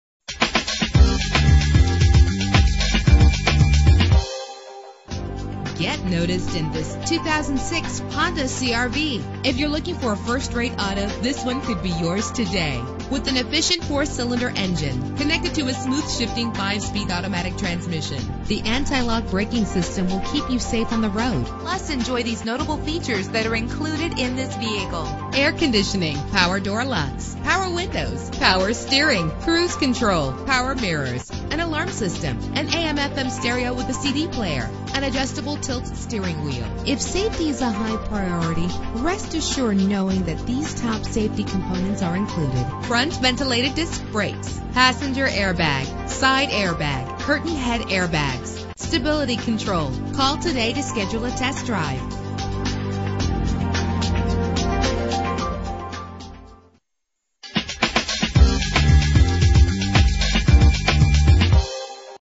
2006 Honda CR-V - Denville NJ
Year: 2006
Make: Honda
Model: CR-V
Trim: LX
Engine: 2.4 liter inline 4 cylinder 16 valve
Transmission: 5-Speed Automatic
Color: Silver
Mileage: 57072
Address:
3166 Route 10